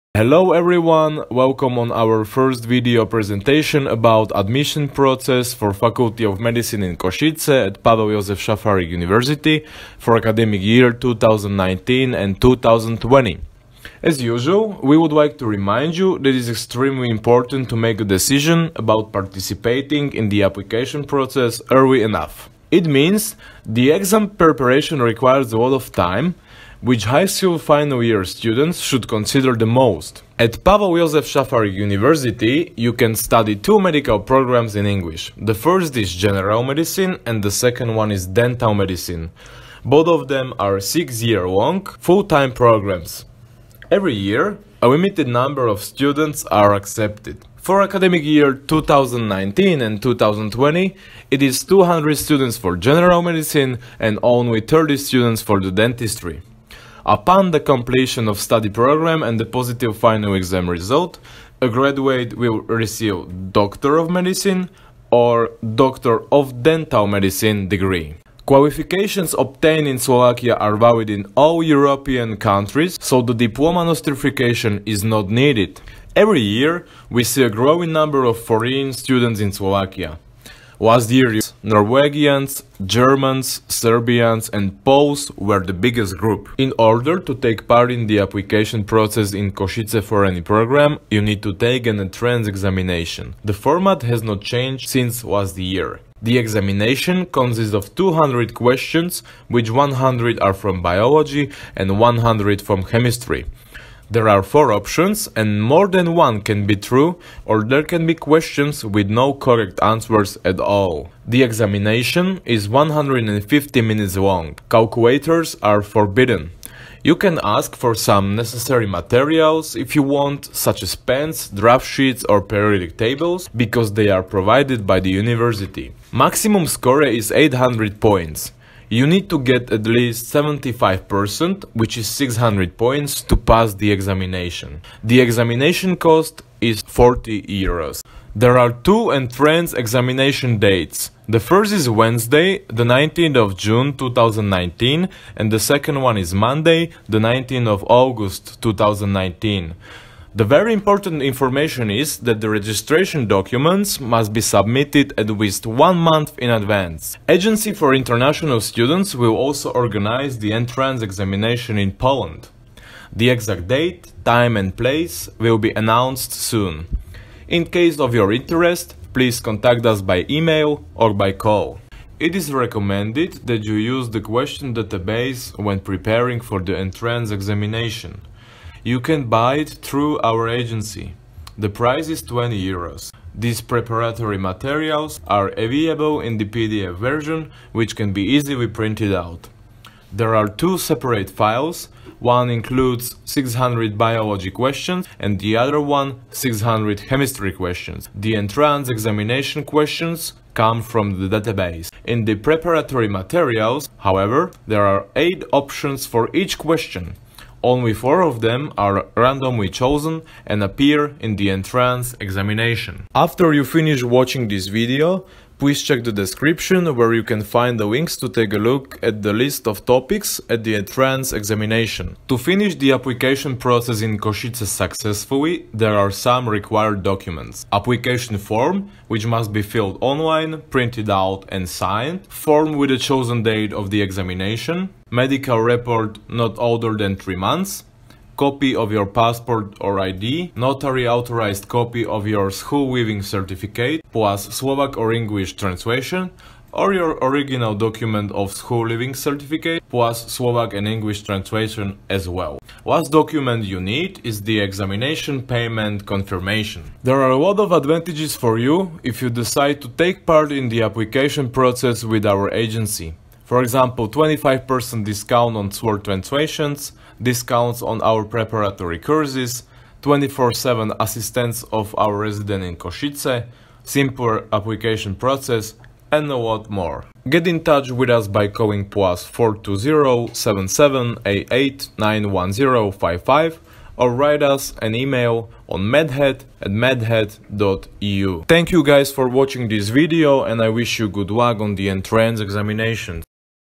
Admission process for Faculty of medicine UPJS in Kosice, Slovakia
Do you plan to start studying programs of General medicine or Dental medicine at University of Pavel Jozef Safarik University in Kosice, Slovakia? We have prepared a video describing all the needed details about the admission process, entrance exams or booklets for the Faculty of medicine in Kosice.